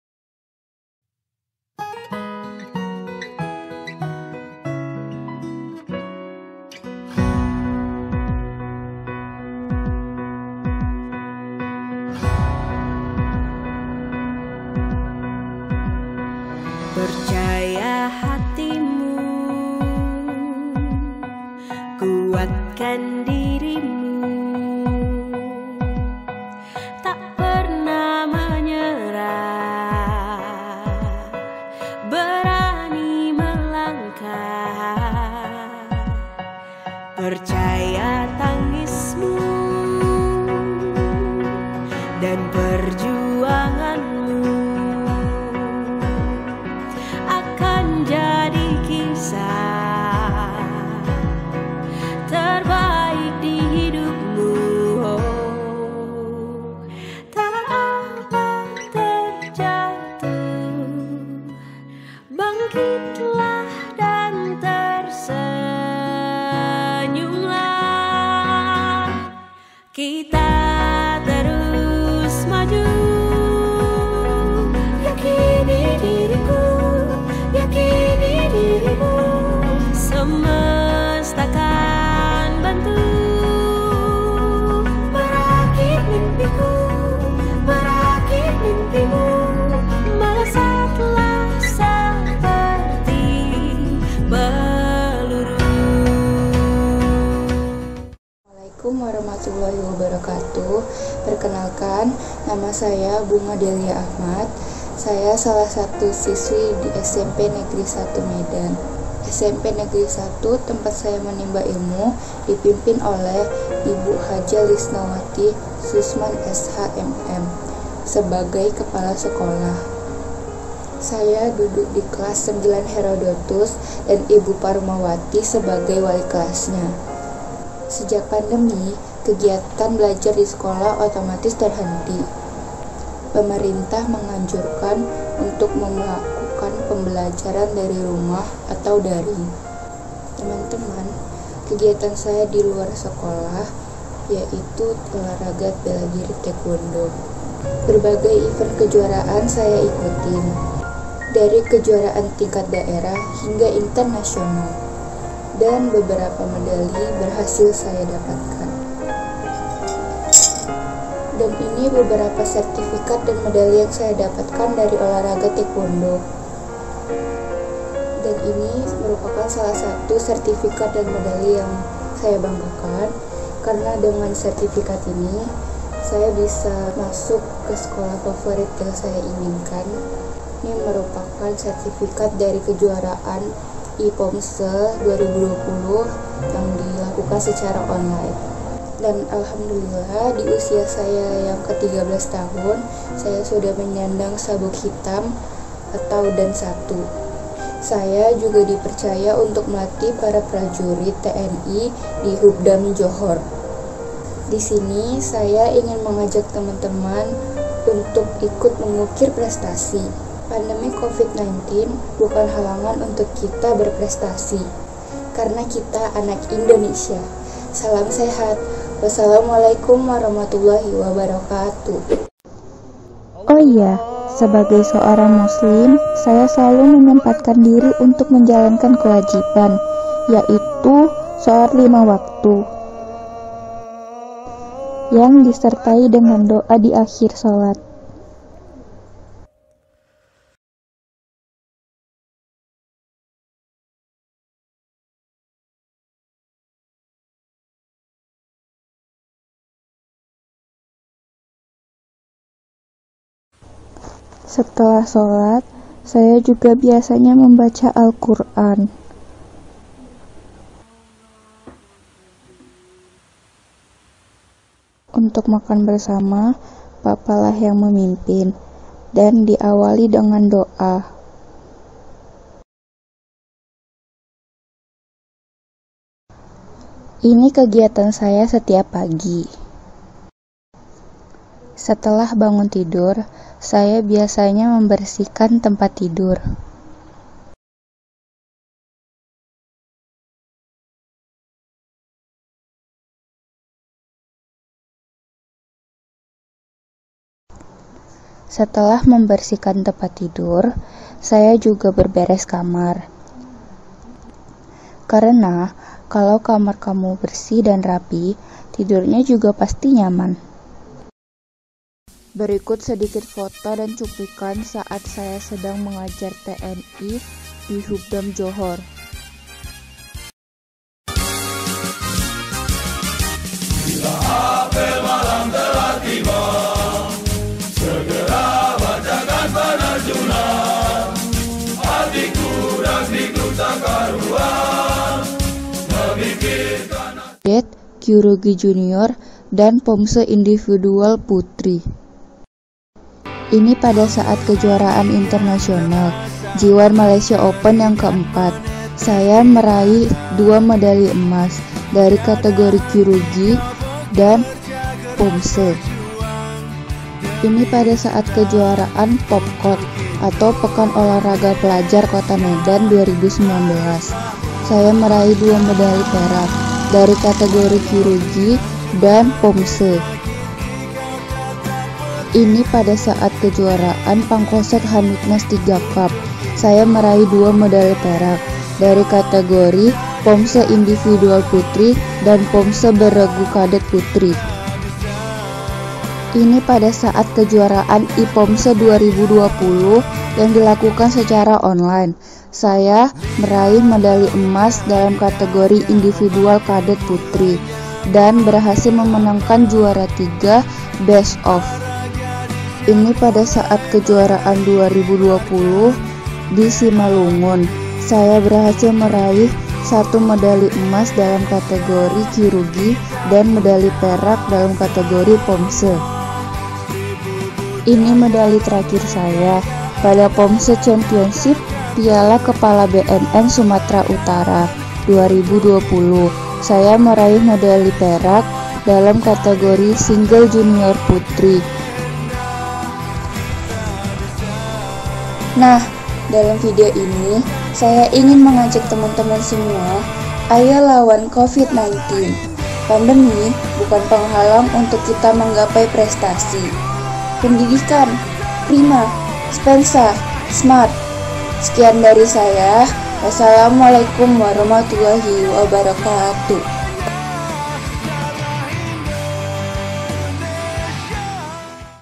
Bunga Adelia Achmad
Video ini dibuat untuk mengikuti Lomba Festival Siswa Berkarakter Kota Medan 2020 yang dilaksanakan oleh Disdik Kota Medan. Jadi mari kita dukung bersama dengan tetap mematuhi Protokol Kesehatan!!!
Mohon like nya ya rekan-rekan dan teman-teman semuanya!
Pendidikan Prima!!!
Spensa Smart!!!
Terimakasih!